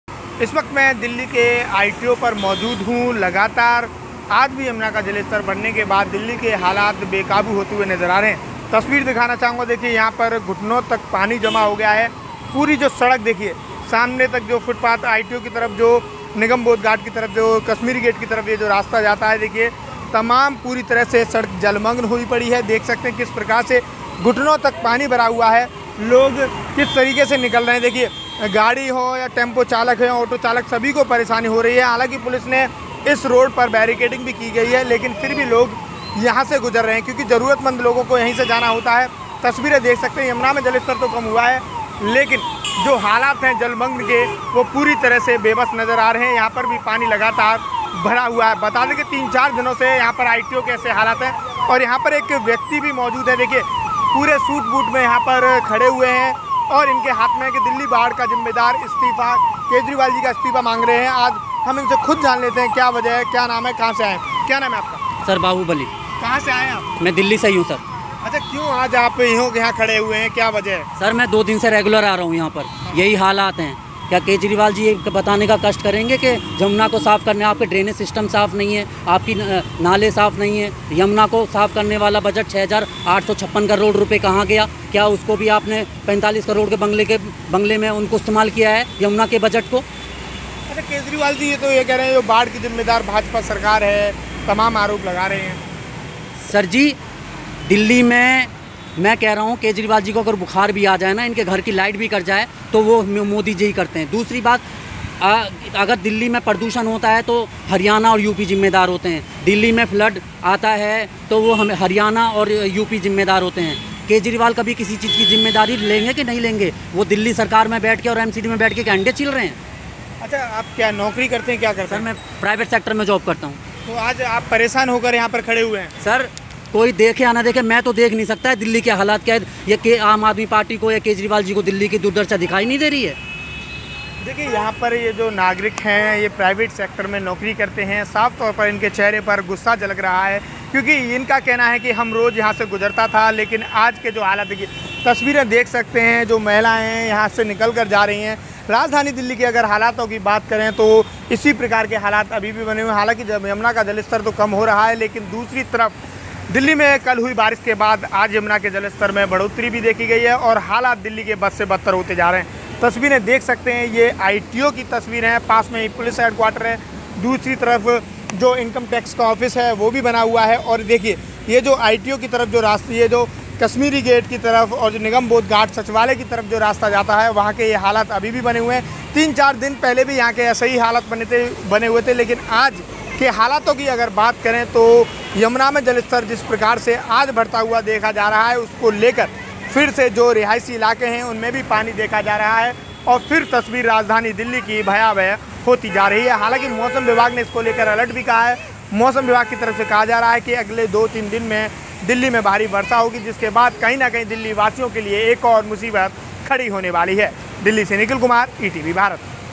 Flood: ITO में पानी में खड़े होकर शख्स ने अनोखे अंदाज में जताया विरोध, CM केजरीवाल से मांगा इस्तीफा
राजधानी दिल्ली के आईटीओ के पास पूरी सड़क बाढ़ में डूबी हुई है. यमुना के जलस्तर में वृद्धि के कारण यह सड़क पूरी तरह जलमग्न है. इस बीच एक शख्स सरकार के खिलाफ अनोखे तरह से विरोध जता रहा है. शख्स को पूरे सूट-बूट में पोस्टर के साथ पानी में खड़ा देखा गया.